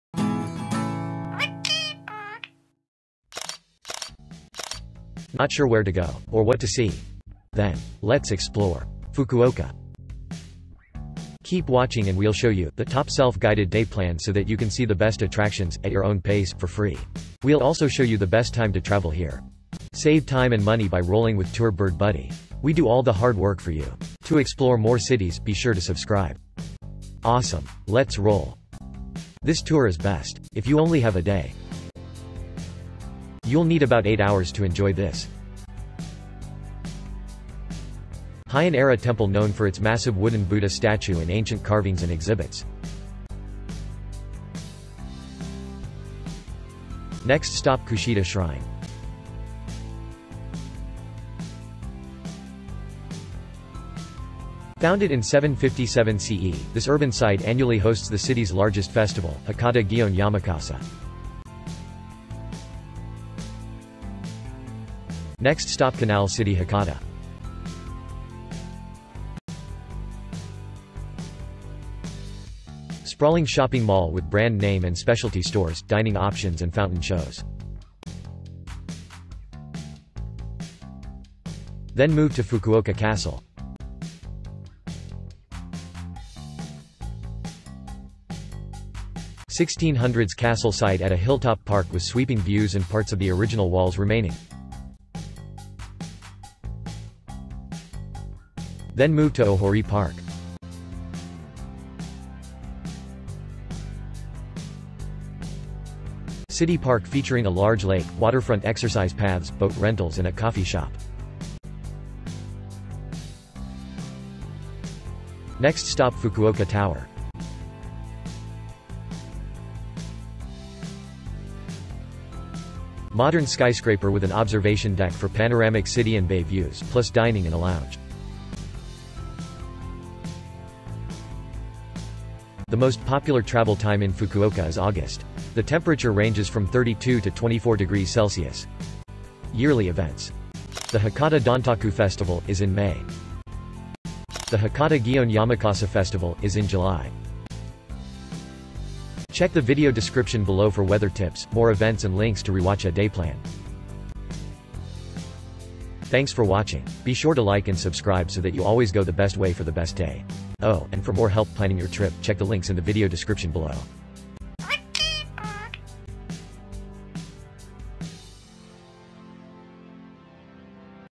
FUKUOKA Japan Travel Guide. Free Self-Guided Tours (Highlights, Attractions, Events)
The top self-guided tour of Fukuoka, Japan. Showing you the best attractions and how to navigate them. Check the links below for further information on how to better plan your journey.

Day Plan Links:
 00:29 - IF YOU ONLY HAVE A DAY

City Description:
Fukuoka, capital of Fukuoka Prefecture, sits on the northern shore of Japan’s Kyushu Island. It’s known for ancient temples, beaches and modern shopping malls, including Canal City. Maizuru Park contains ruins of 17th-century Fukuoka Castle. The central Hakata district contains Tōchō-ji Temple, home to a 10m wooden Buddha and the Hakata Machiya Folk

Weather and Events:
Peak periods include Mar–May and Aug–Dec. Summers are hot and humid and winters are mild, with a rainy season Jun–Sep. The Hakata Dontaku Festival (May) features a costumed parade with traditional music and dance. The Hakata Gion Yamakasa Festival (Jul) sees men racing through the streets carrying giant floats.